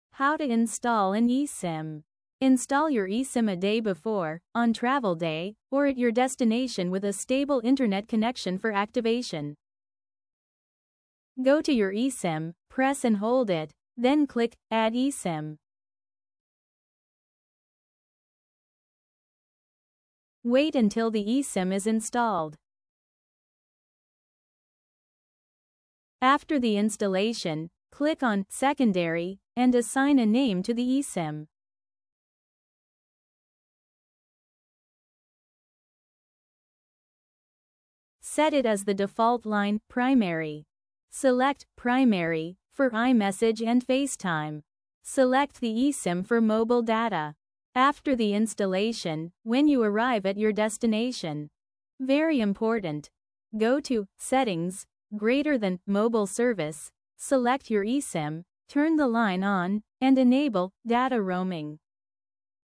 How to Install an eSIM on Your Smartphone – A Step-by-Step Guide | Simbye.com
Learn how to easily install an eSIM on your smartphone with this step-by-step guide. Whether you're setting up an eSIM for the first time or switching from a physical SIM card, this guide will walk you through the entire process. Get connected quickly and hassle-free!